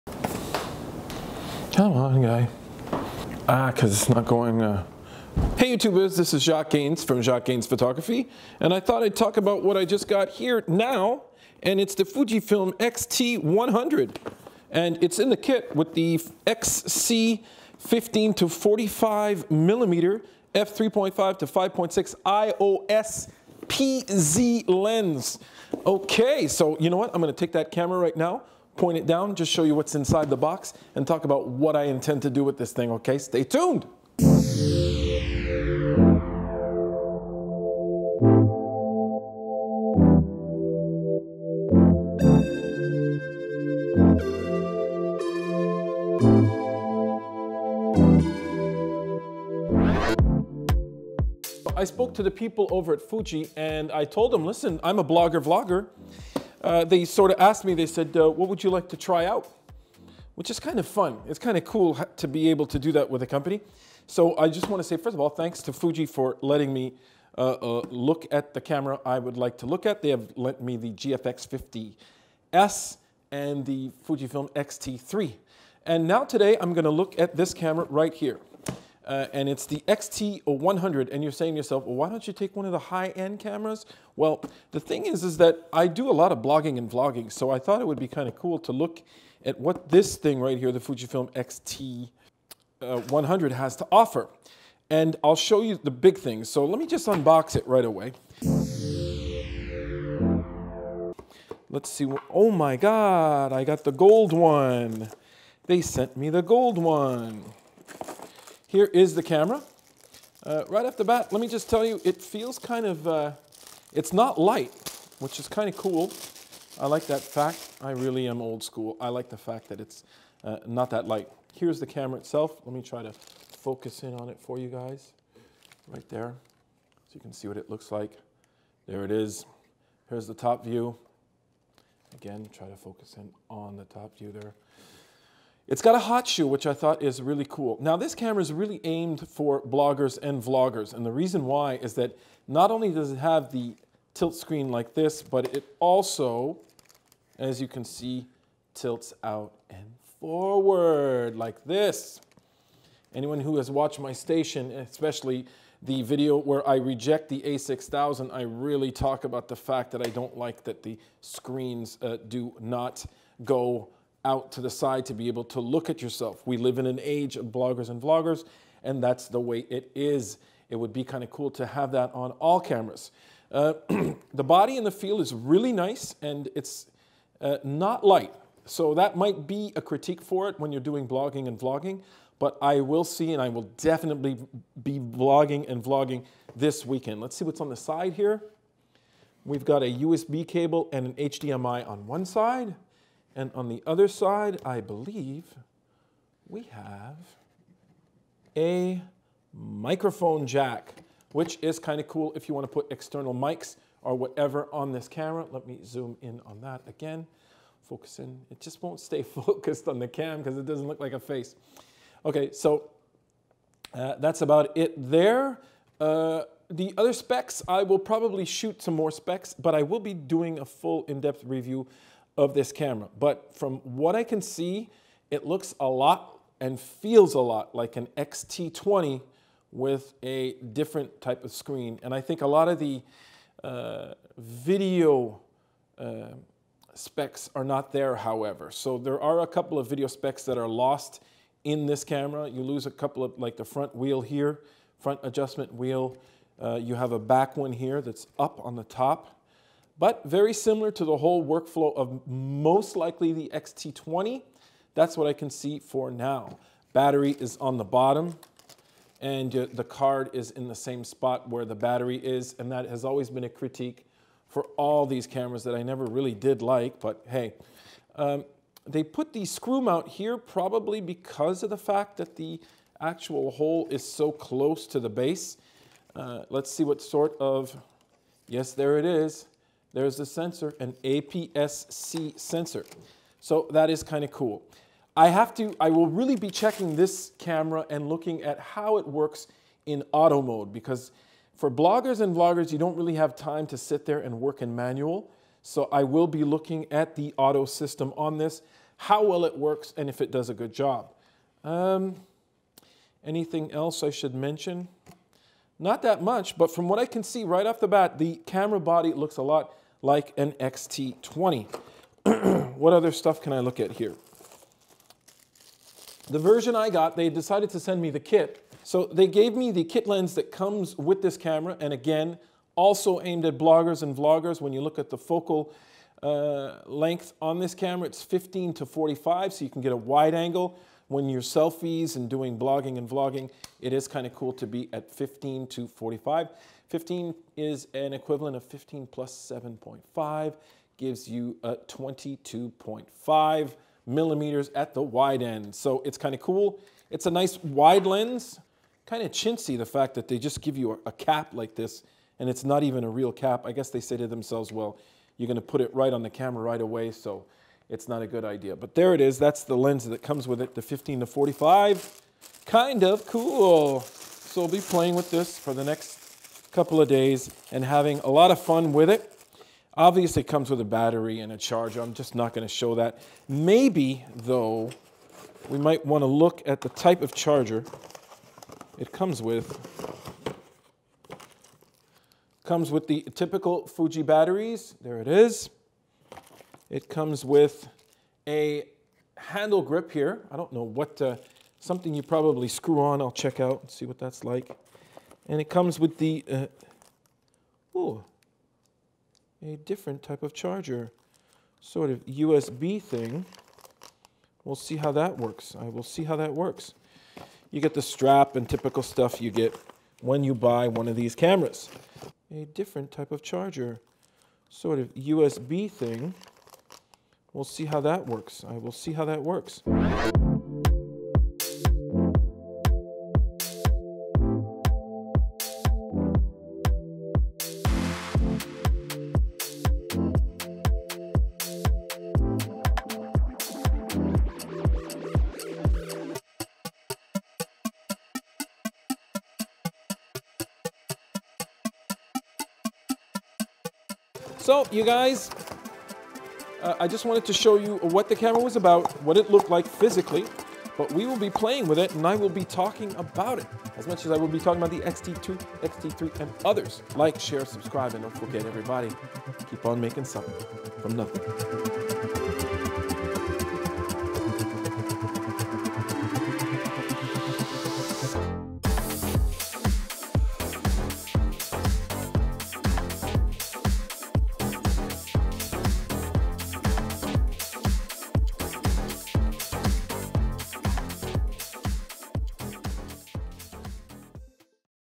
FUJIFILM X-T100 FIRST LOOK. Is it good for vlogging?
Under not too much fanfare, Fujifilm introduced a smaller low cost version of their cameras called the Fujifilm XT100. It is no slouch however. Sporting some really great specs and, with its new rotating screen, it becomes a temptation for those who want to make their pressence on social media. By having a flip screen and a great deal of instant gratification specs it might just be a contender. In this video I look at what is inside the box and talk about the physical appearance of the Fujifilm XT100 and the FUJINON XC15-45mmF3.5-5.6 OIS PZ lens that comes with the kit.

If you would like to buy some of my T-shirts, hoodies, and tank tops, click on this link